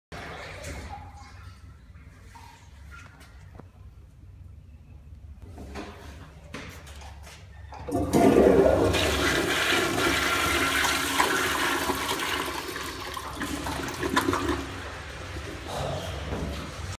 325. Vintage American Standard Afwall Toilet.
I really love this toilet. This toilet has an amazing flush. This video was filmed at a visitors center at a park.